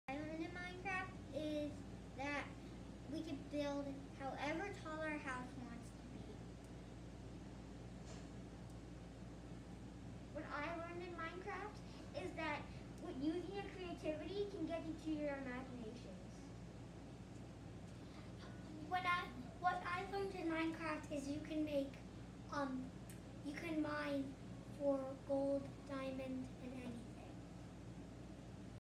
Minecraft Survival for Beginners
Campers reflect on their week of Minecraft adventures on the last day of camp